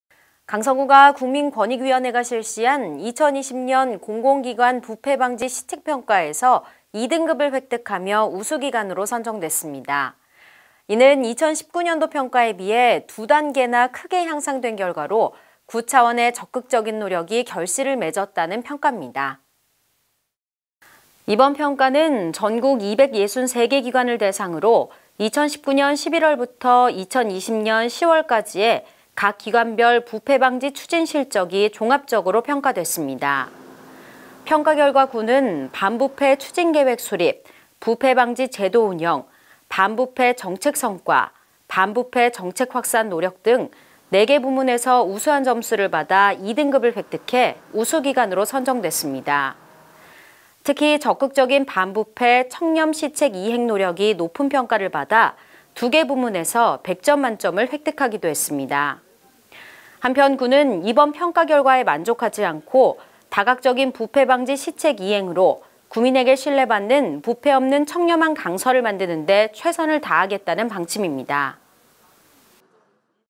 부패방지 시책평가 우수기관 선정
강서구가 국민권익위원회가 실시한 ‘2020년 공공기관 부패방지 시책평가’에서 2등급을 획득하며 우수기관으로 선정됐습니다.
이는 2019년도 평가에 비해 두 단계나 크게 향상된 결과로 구 차원의 적극적 노력이 결실을 맺었다는 평가입니다.

이번 평가는 전국 263개 기관을 대상으로 2019년 11월부터 2020년 10월까지의 각 기관별 부패방지 추진 실적이 종합적으로 평가됐습니다.
평가 결과 구는 반부패 추진계획 수립, 부패방지 제도 운영, 반부패 정책성과, 반부패 정책 확산 노력 등 4개 부문에서 우수한 점수를 받아 2등급을 획득해 우수기관으로 선정됐습니다.
특히 적극적인 반부패·청렴 시책 이행 노력이 높은 평가를 받아 2개 부문에서 100점 만점을 획득하기도 했습니다.
한편 구는 이번 평가 결과에 만족하지 않고 다각적인 부패방지 시책 이행으로
구민에게 신뢰받는 부패 없는 청렴한 강서를 만드는데 최선을 다하겠다는 방침입니다.